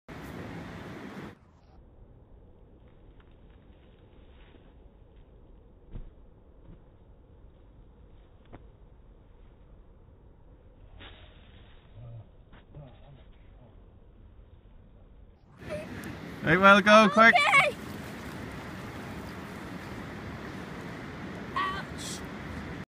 Dune jumping
Jack launches off the Murlough Dunes